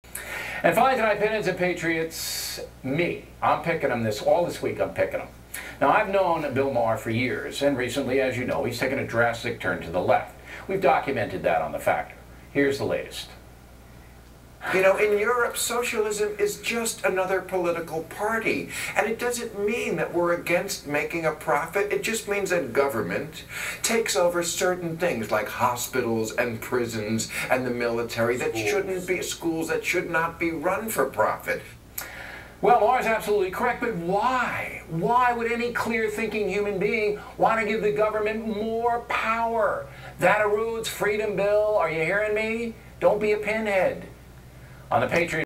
Bill O'Reilly: Bill Maher Defends Socialsim
Maher is such a sniveling little weasel. Socialism is far more than just taking over schools and hospitals; it is about empowering the government to decide wealth so power can be more greatly concentrated on government that is why the socialists just got ran out of Spain the other day. Socialists like Maher are such rank hypocrites, oh they love their own wealth, their big homes, fancy cars, wine and dine lifestyle, yet condemn the very system that allowed them to obtain it in the first place. Go check out Maher's home one time, or see how fellow socialist Michael Moore really lives. They are devout capitalists with their money, but socialists with everyone else's.
Socialism, like its angry big brother communism, preaches total economic equality, meaning no upper class or lower class. Would Maher really like to live in a socialist state? Go ahead Maher, move to Venezuela, or Denmark, watch those riches you love so much go to the government you and your fellow zealots want to empower